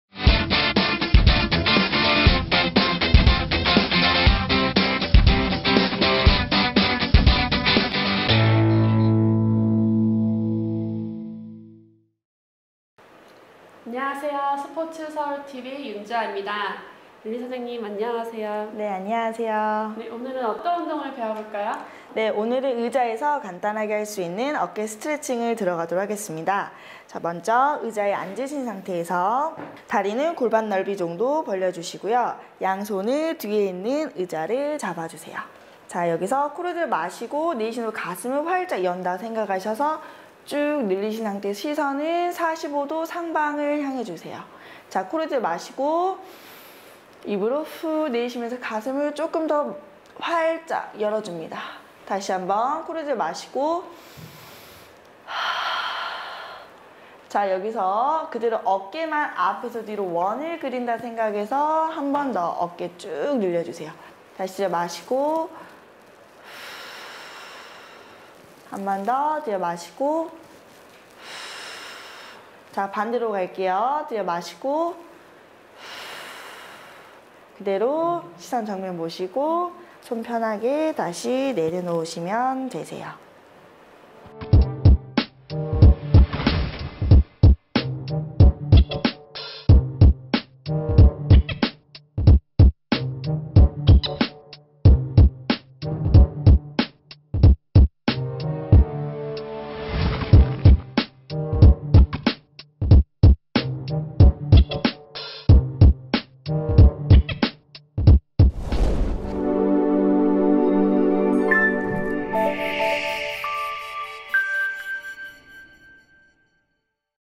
[윤주하의 보디보디] 직장인을 위한 어깨와 가슴 스트레칭
[스포츠서울닷컴ㅣ김동준 기자] 장시간 책상에 앉아 사무에 집중하는 직장인, 또 수험생을 위한 스트레칭은 어떤 게 있을까? '스포츠서울닷컴'은 매일 독자들의 건강을 위한 스트레칭을 제안한다.

[윤주하의 보디보디]가 제안하는 스트레칭은 컴퓨터 작업이 많은 직장인을 위한 어깨와 가슴 운동법이다.

먼저 의자에 앉은 상태서 양손으로 뒤에 있는 의자를 잡고, 호흡 들이마시고 내쉬며 가슴을 활짝 펴면서 시선은 하늘을 향한다.
가슴을 활짝 펴서 가슴 앞쪽이 최대한 스트레칭 될 수 있도록 늘려준다.

의자에 바르게 앉아 시선은 정면을 향한 상태서 어깨만 앞에서 뒤로 원을 그리듯 돌려주면서 어깨 스트레칭을 10회씩 반복한다.